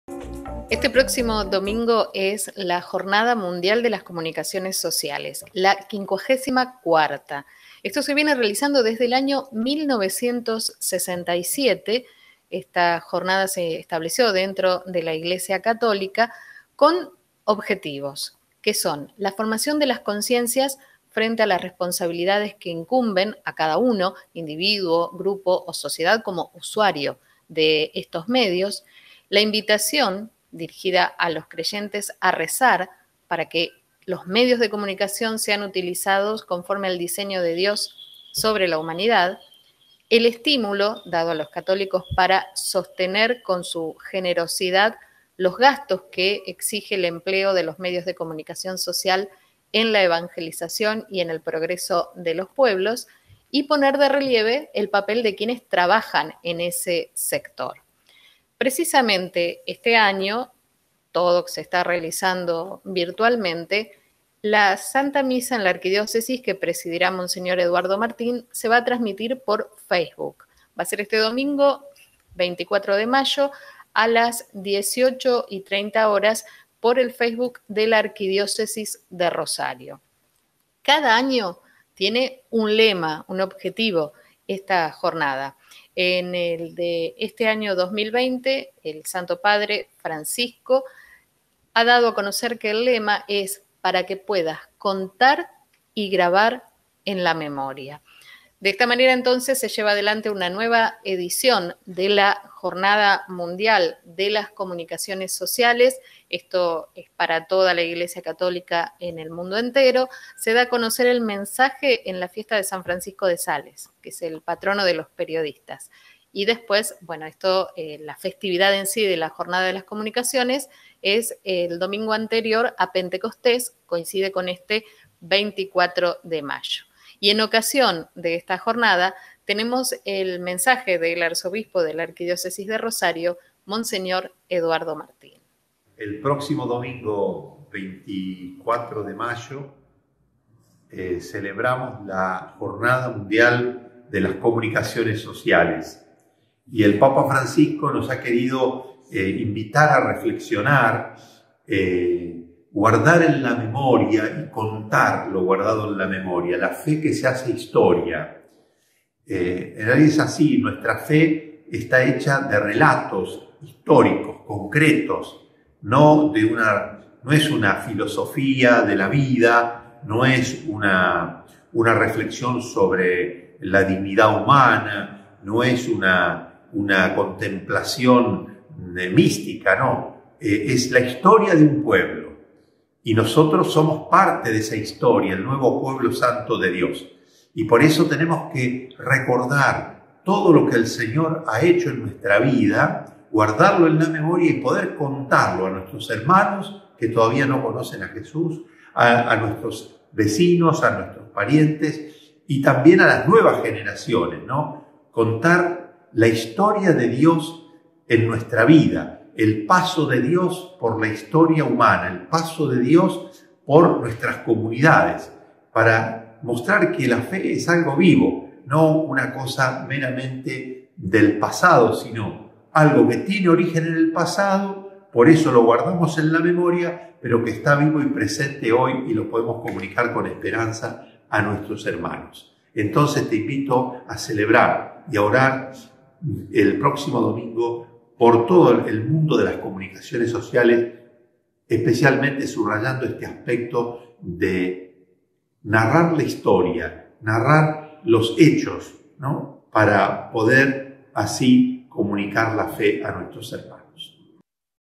54ª Jornada de las Comunicaciones Sociales
Monseñor Eduardo Martín, arzobispo de Rosario, y la invitación a participar de la 54ª edición de la Jornada de las Comunicaciones Sociales.